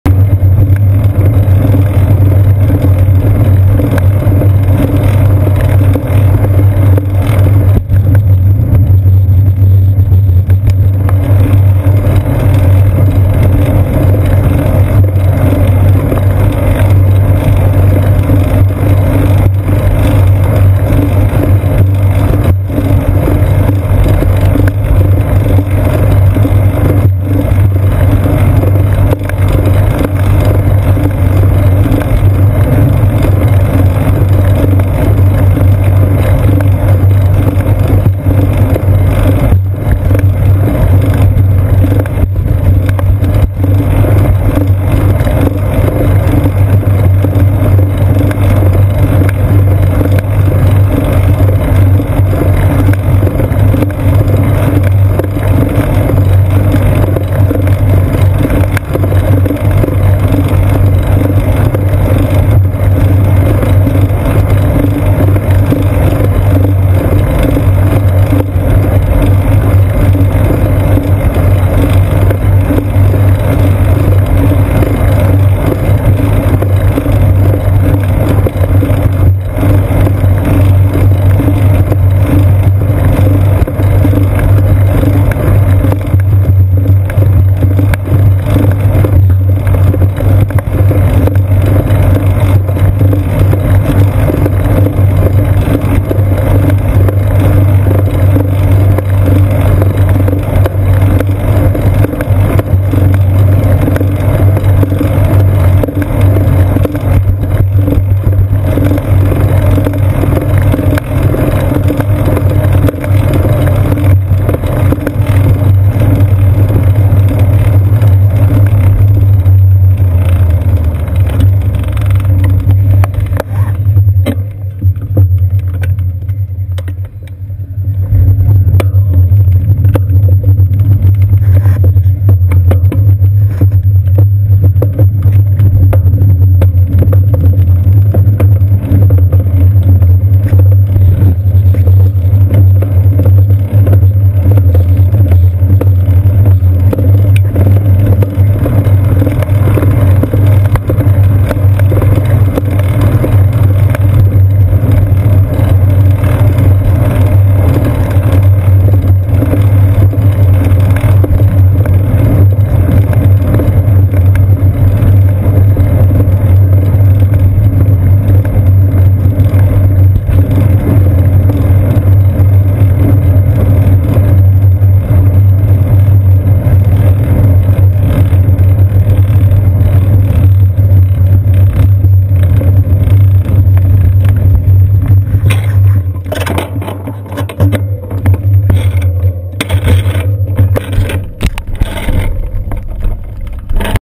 Florida Bike commute Wednesday morning - Quiet and sunny - rear camera, part 1/2
Although still humid (right?!?!?) here in Florida the sun is back this morning and it feels quiet while biking through the streets of Safety Harbor and Clearwater, Florida on the way to work on the morning of June 24, 2015.